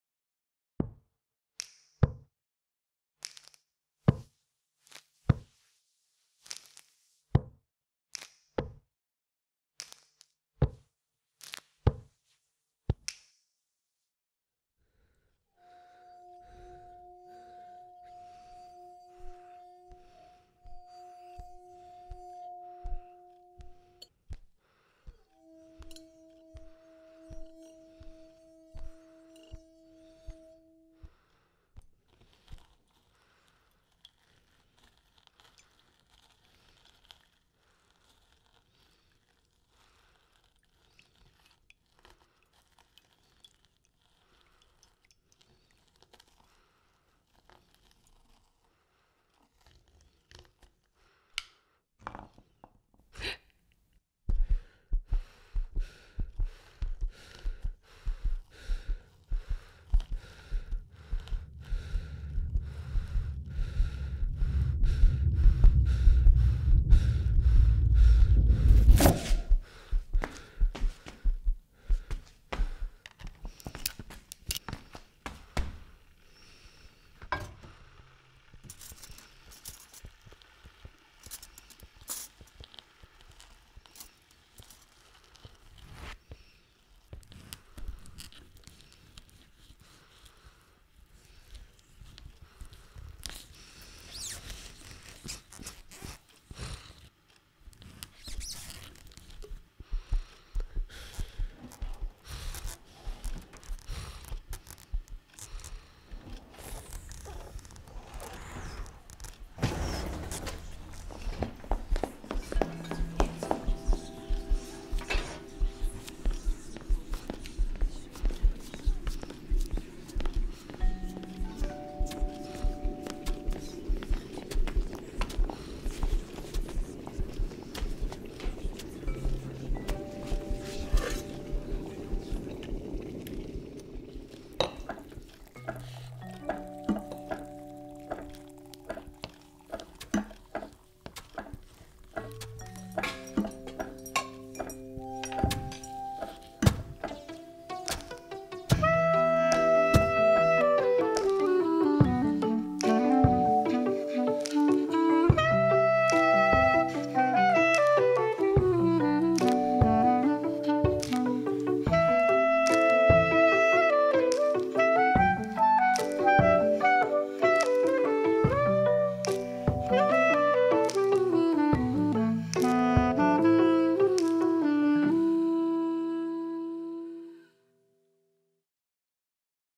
solivagant (stop motion animation) (immersive audio)
**PLEASE LISTEN ON HEADPHONES FOR THE FULL IMMERSIVE EXPERIENCE**

Solivagant, noun or adjective for "solitary wanderer". A tiny snail on an adventurous detour. Hearts beating, astonishment, sense of rhythm. At a wedding buffet.

Graduation film for my Bachelor studies - one-woman-project, no budget.

Go to thatparanoidsnail.tumblr.com/ for the story behind or get inspired and create your own snail cosmos like my friend did